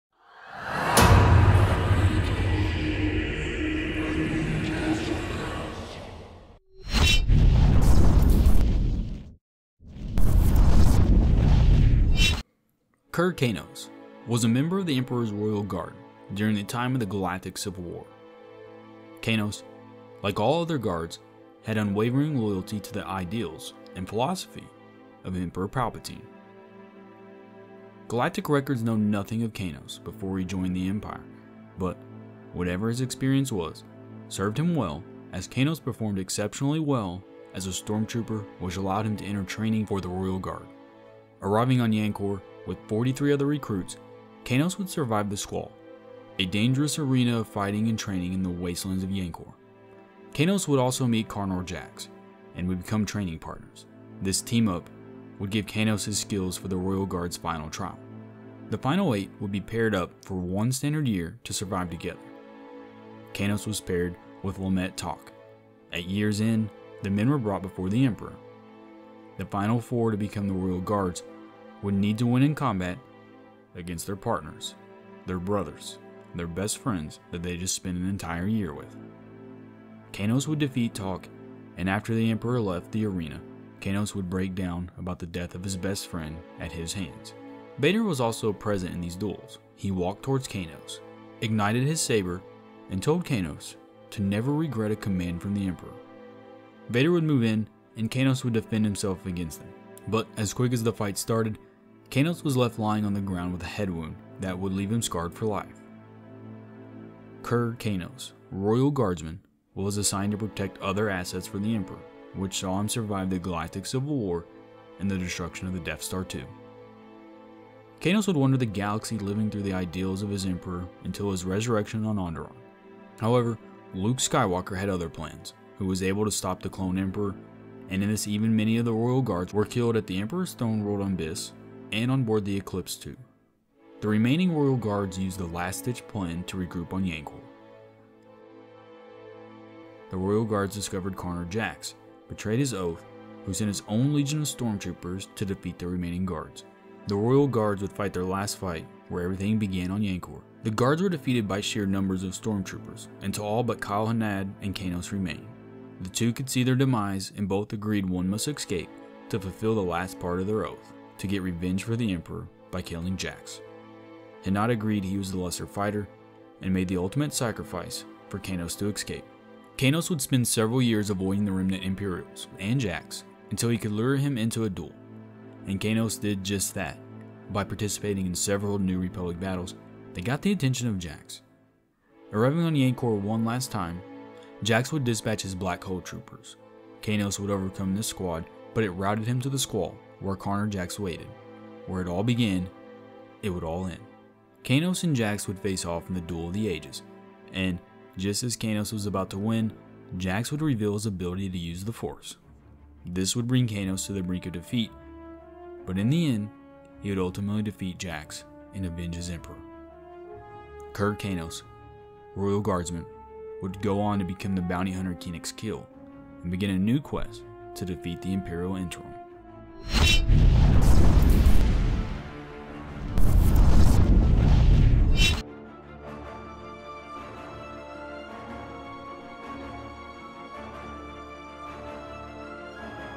Who is Kir Kanos? Star Wars Explained
In this episode of Star Wars Quick Clips we cover Kir Kanos. Not to be confused with Carnor Jax his arch rival. Find out all you need to know about this Royal Guard and new Hasbro Black Series figure!

bensounds . com for the music!!